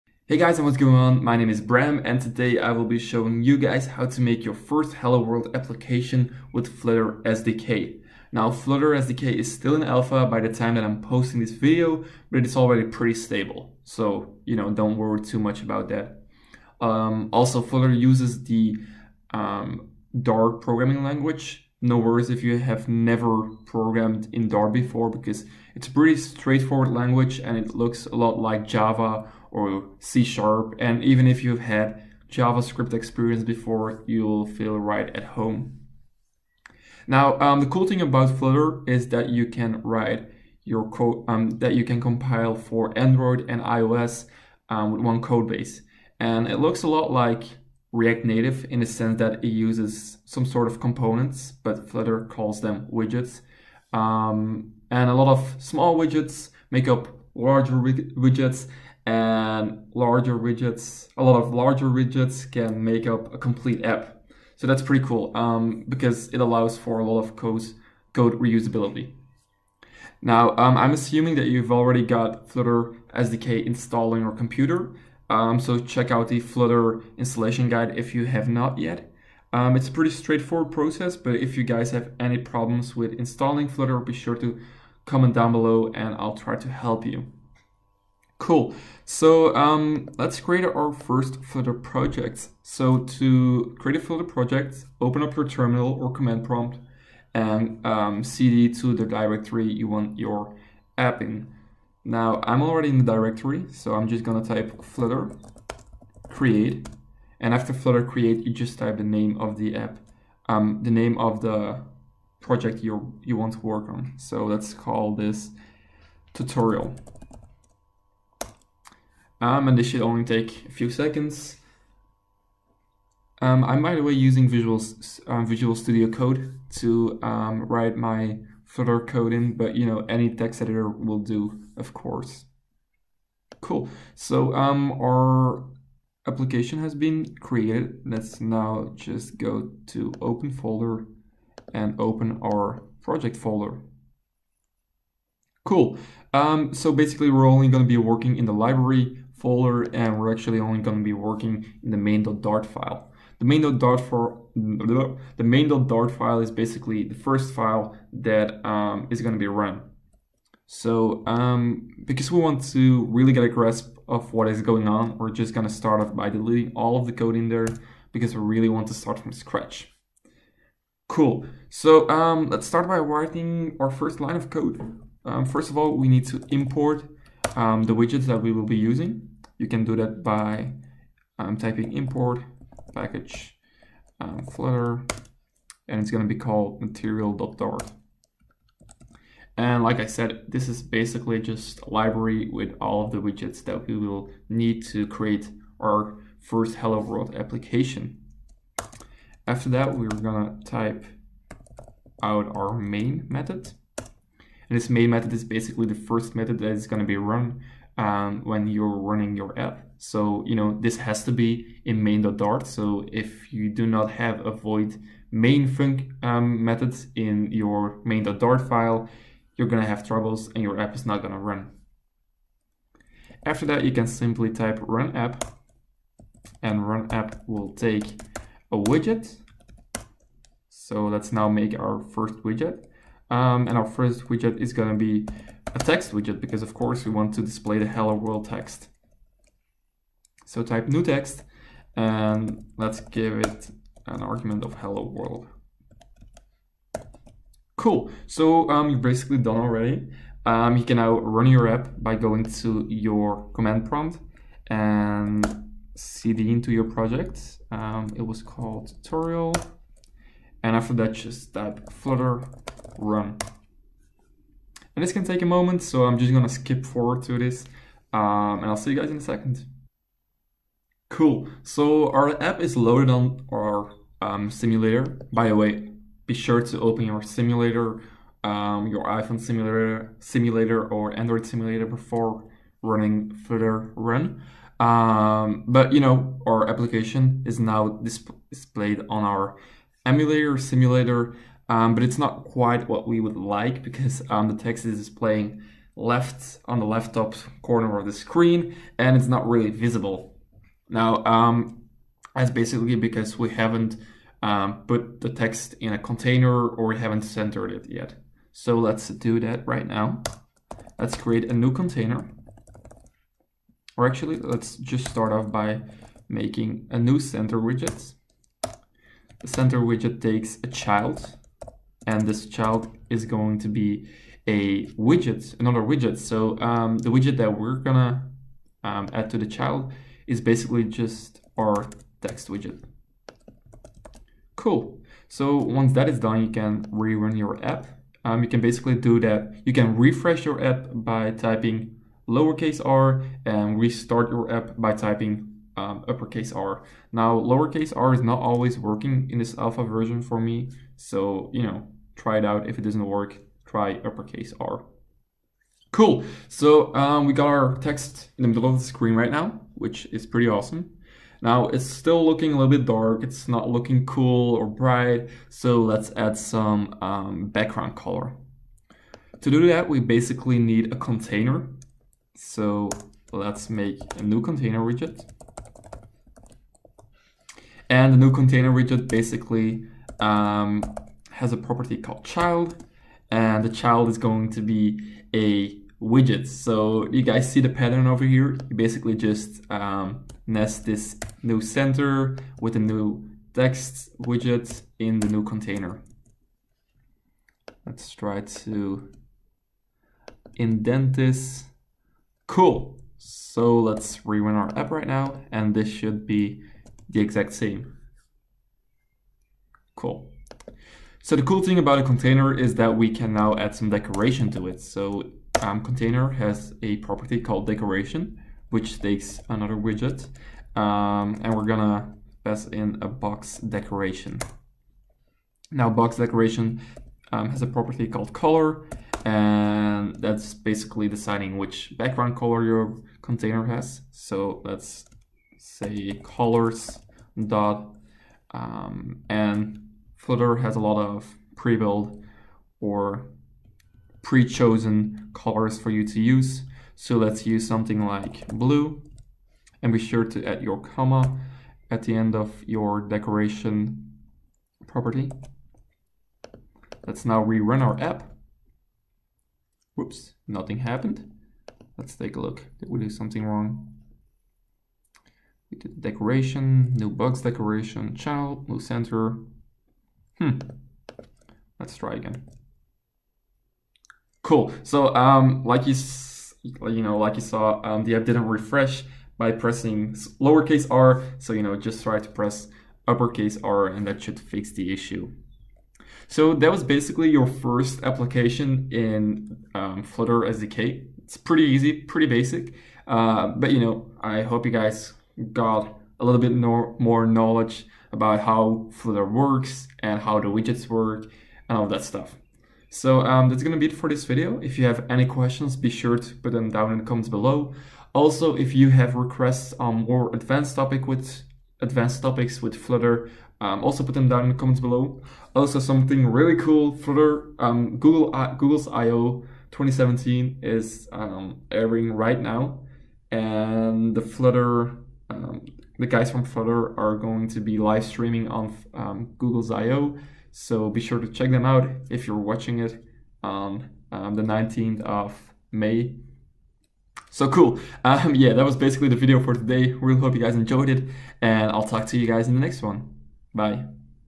Flutter SDK Tutorial - Hello World! (App Development)
In this tutorial, I guide you through coding a Hello World application for both iOS and Android with the Flutter SDK. This tutorial is for  beginners, so I only cover basic stuff! :)

NOTE: I'm using the android simulator, but you can simply open an iPhone simulator to run the same code!

Be sure to ask for help in the comments if you need any help. Suggestions for future Flutter tutorials are also very welcome! :D

Flutter is an SDK owned by Google to create applications for Android and iOS using a single codebase. Flutter uses the Dart programming language (also owned by Google). Flutter was in alpha when I released this video, so things might still change a bit. But the SDK is already mature enough to write some cool apps!